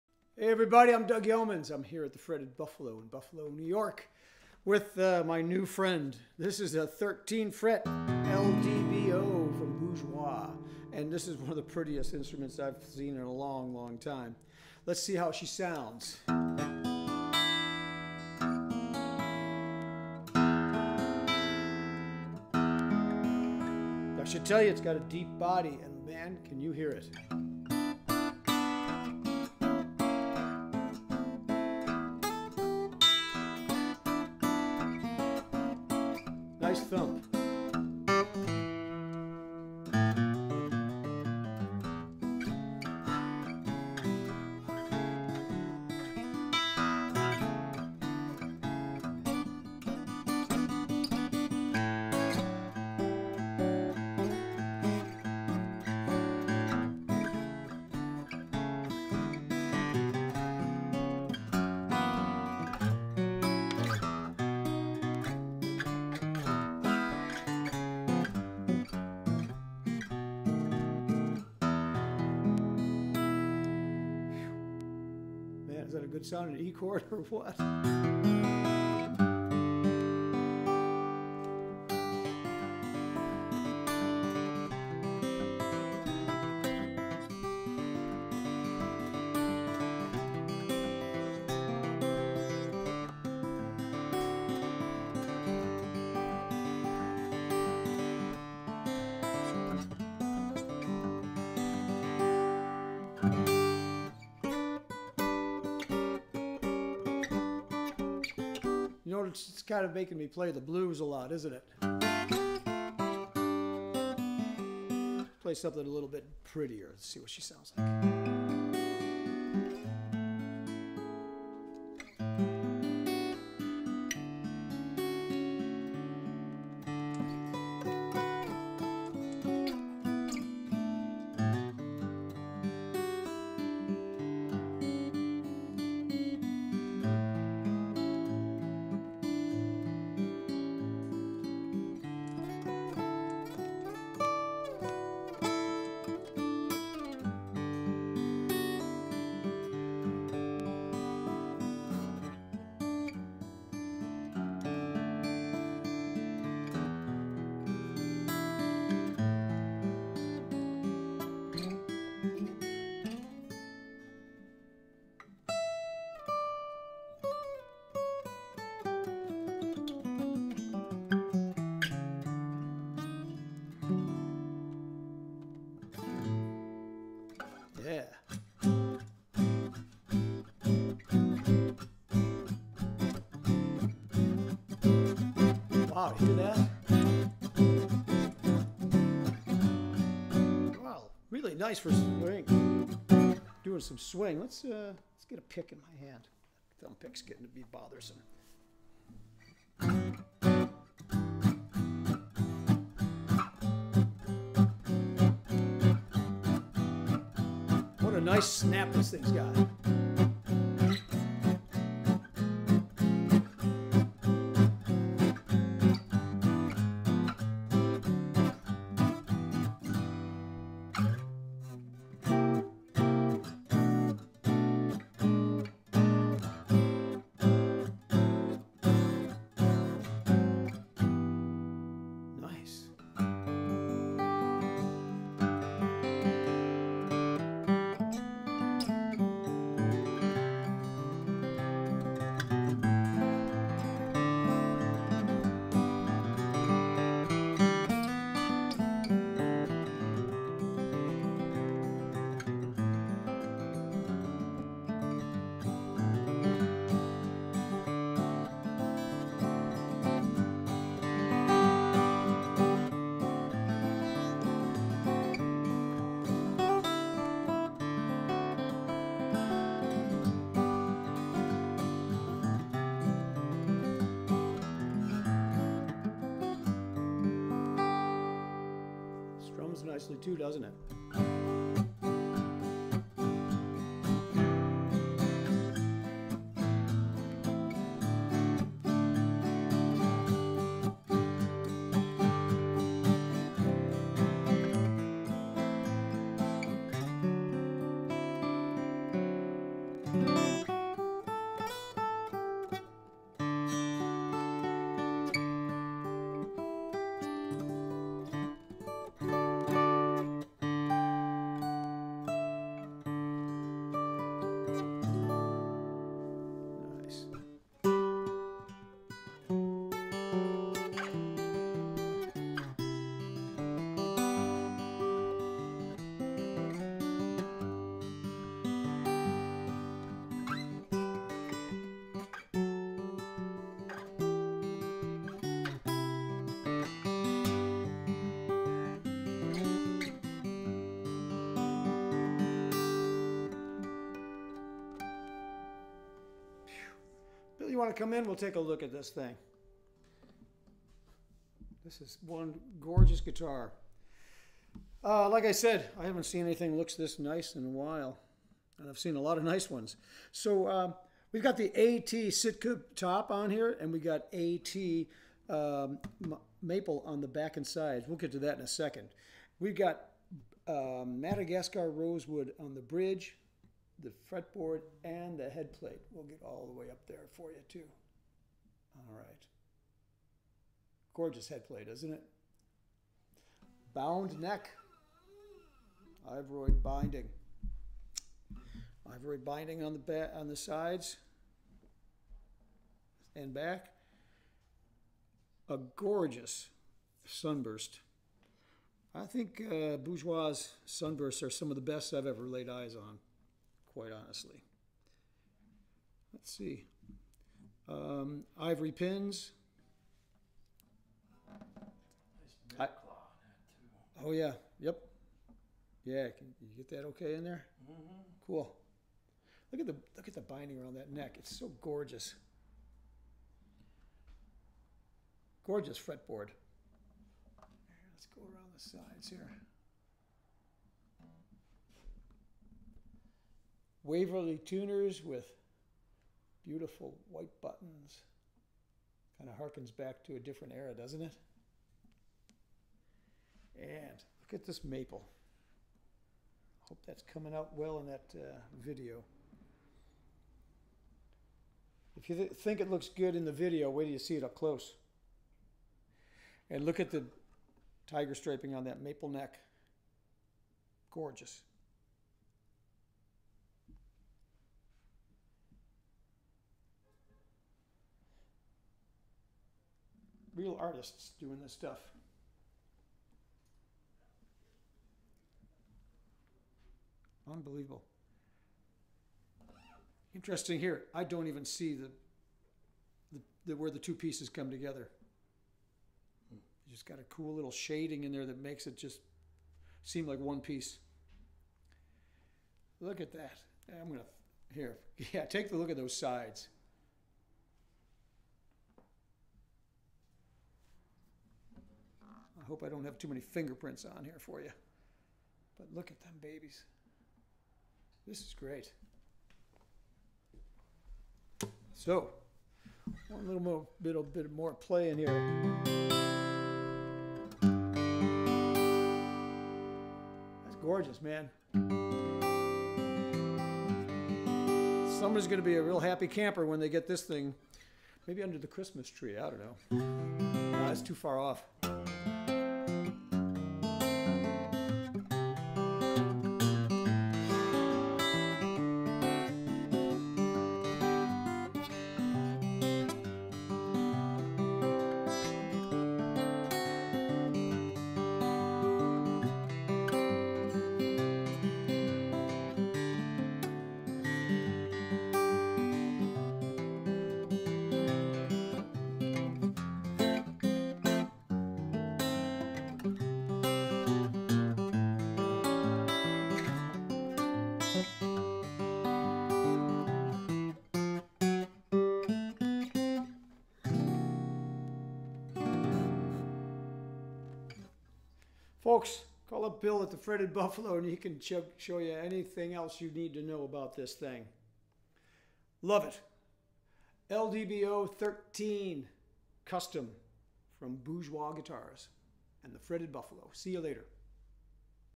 Bourgeois LDBO 13 Custom Luthiers Choice Bear Claw Sitka / Flamed Maple
Our thanks to our good friend Doug Yeomans for the demonstration video! For more information and pricing on this guitar, please